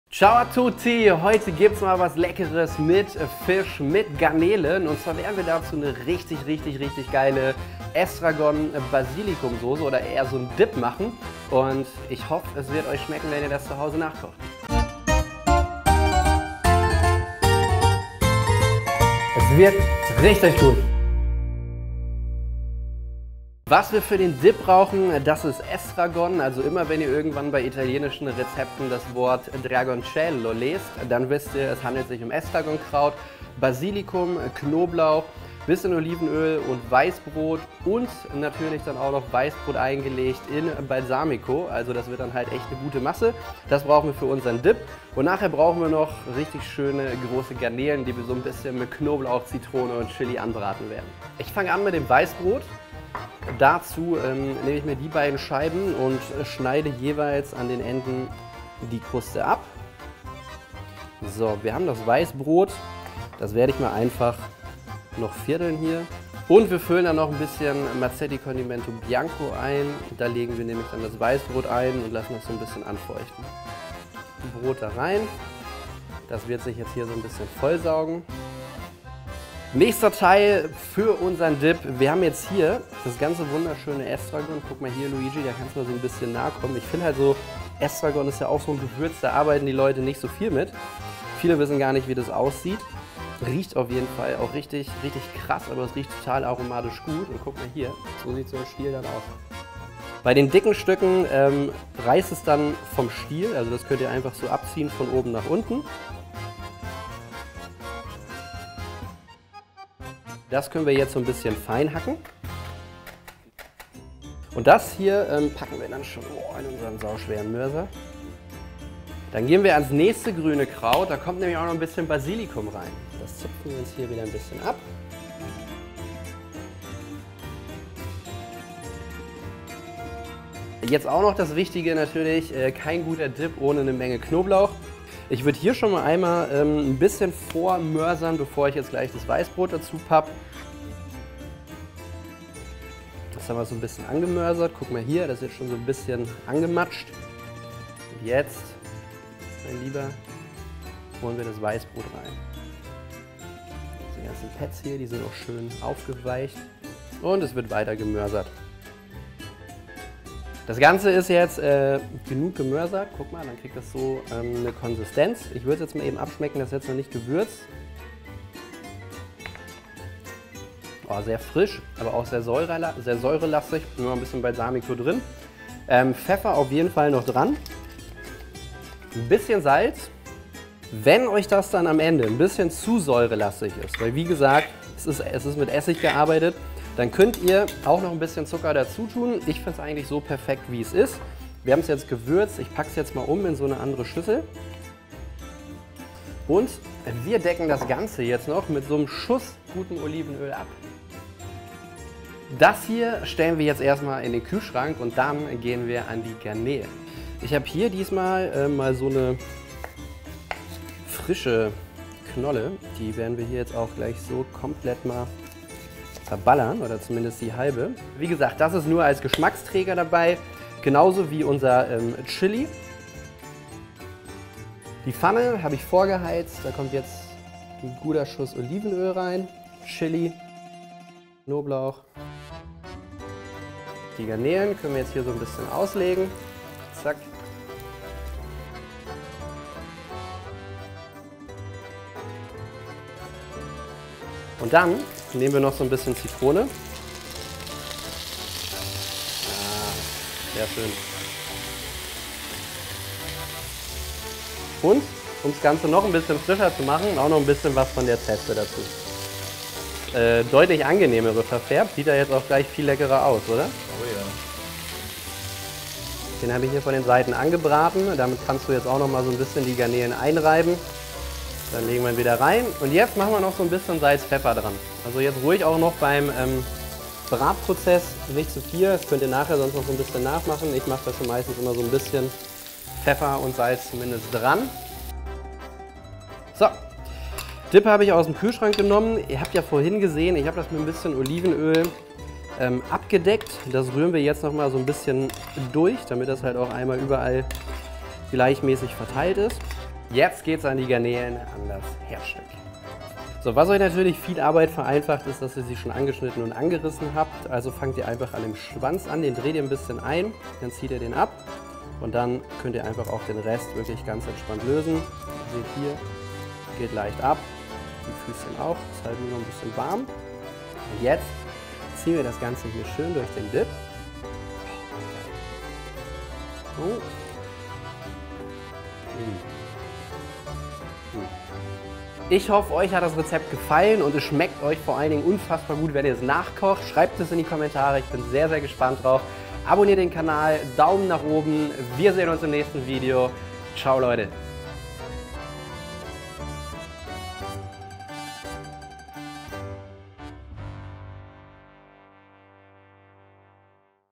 Garnelen mit Estragonsauce
Dass unter euch viele Seafood-Fans sind, wissen wir ganz sicher!
Aus diesem Grund & weil Meeresfrüchte einfach immer eine gute Idee sind, hält Giuseppe in dieser Woche Garnelen mit Estragonsauce für euch bereit.
Wie euch dieses Gericht in nur 35 Minuten Vorbereitungszeit satt & glücklich macht, verrät euch maestro Cotrufo.